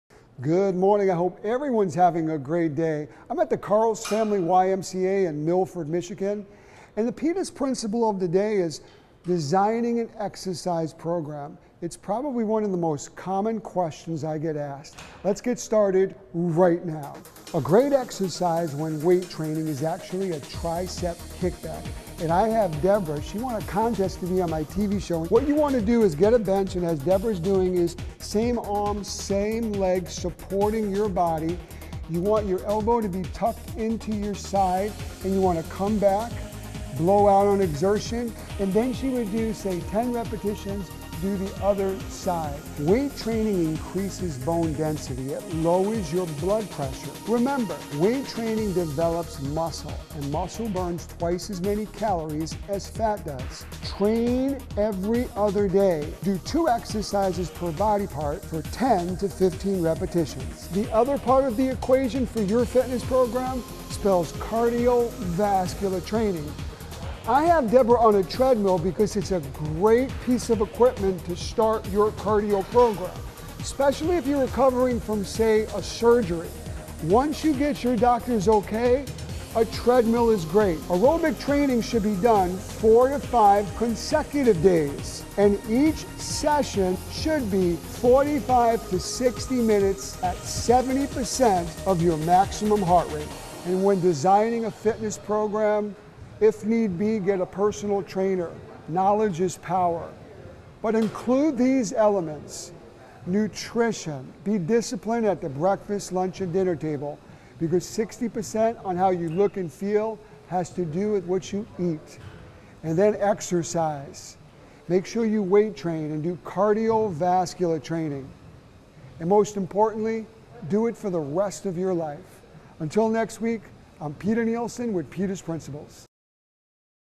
Peter's Principles - Starting Your Workout Program (Dunham's Sports)
Fitness expert Peter Nielsen discusses how nutrition and exercise (weight & cardio training) are the building blocks to designing your own exercise program.